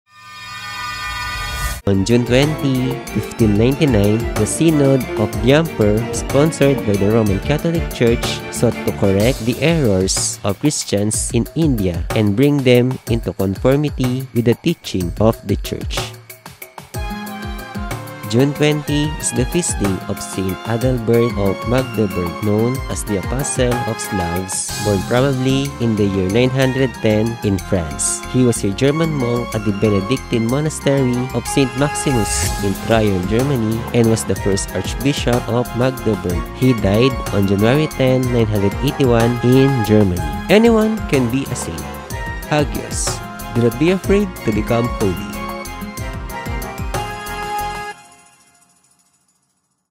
Hagios : Saint of the Day ( June 20 ) - Saint Adalbert of Magdeburg
Saint Adalbert of Magdeburg, known as the Apostle of the Slavs, born probably in the year 910 in France. He was a German monk at the Benedictine Monastery of Saint  Maximinus in Trier, Germany and was the first Archbishop of Magdeburg. He died on June 10, 981 in Germany.

Trivia
On June 20, 1599 the Synod of Diamper, sponsored by the Roman Catholic church, sought to correct the errors of Christians in India and bring them into conformity with the teachings of the church.